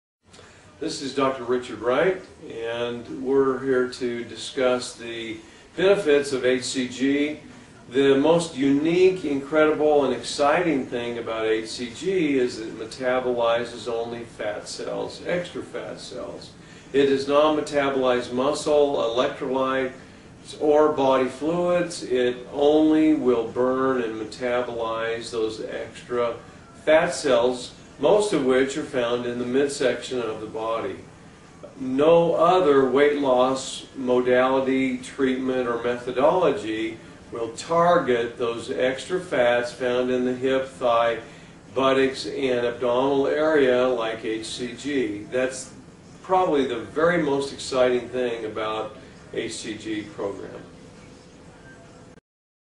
HCG diet Scam!! Does HCG really work?? Is HCG bad for you?
I would LOVE it if there was a diet that actually works!! Isn't that what everyone really wants? Of course we know that eating less and moving more is something that works, but who wants to do that??